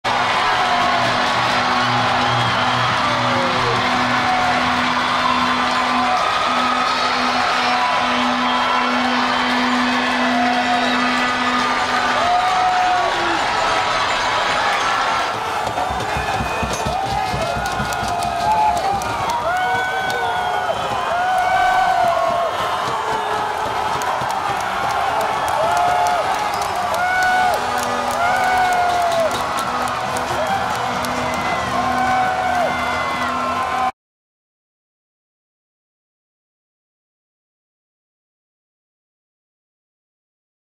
Stratford Warriors end of the game celebrations as the buzzer sounds
The Warriors take home the Cherrey Cup title in front of a huge crowd at the Allman Arena on Friday April 7th.

This is the end of game excitement. Much more to come on 519.